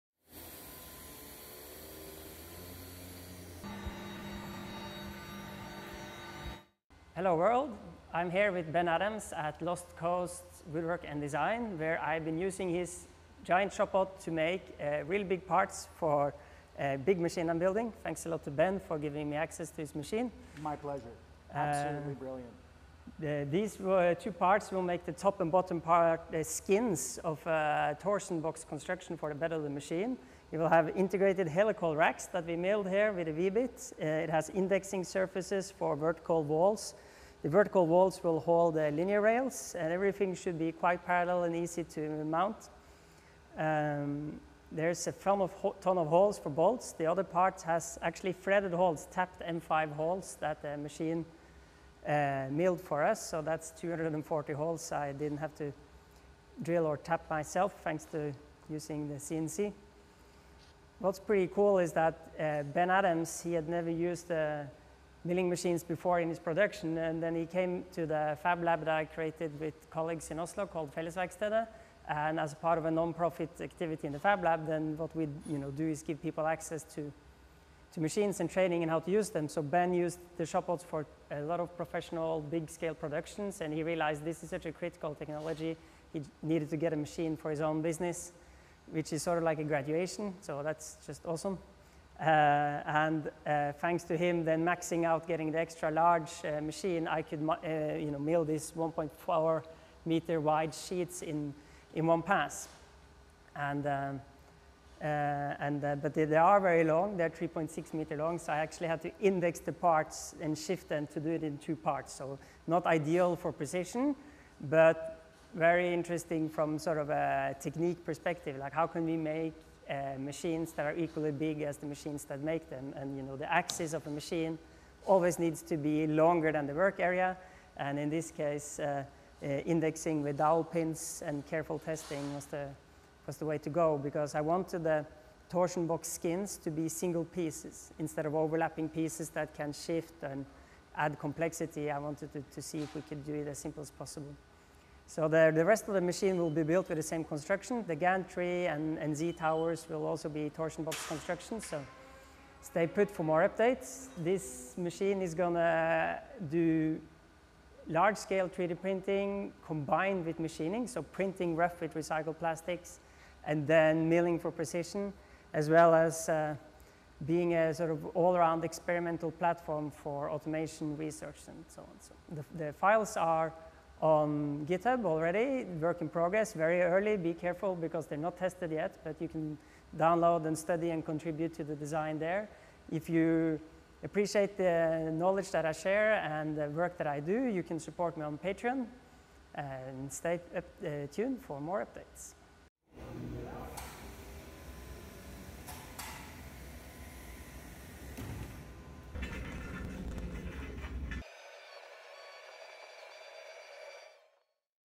Building a giant 3D printer and mill part1 - milling a torsion box bed
I am designing and building Hanzo, my dream digital fabrication research machine. The large cartesian frame will 3D print with recycled plastics and CNC mill for precision.

Designed in Freecad, milled with my own open software Bark Beetle for Rhino

A big thank you to Ben Adams at Lost Coast Woodwork and Design!